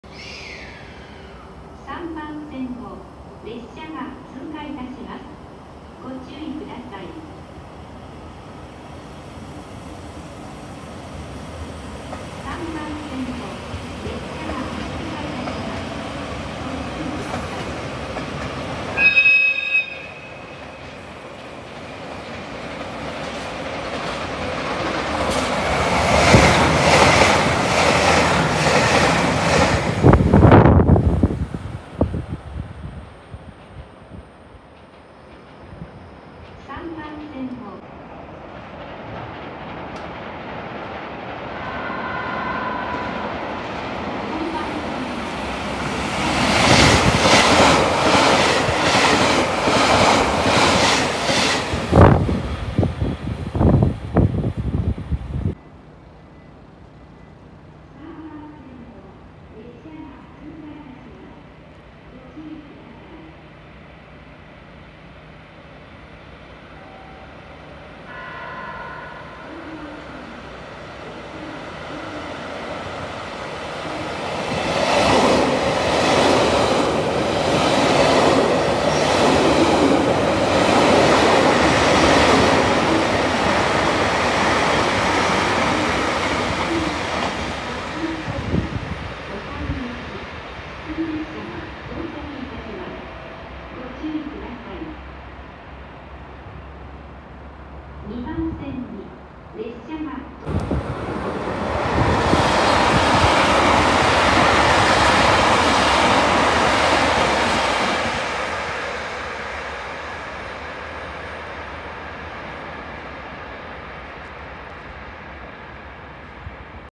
朝8時台の苗穂駅で30分ほど通過列車を
四季島を撮影したついでに苗穂駅に行きました。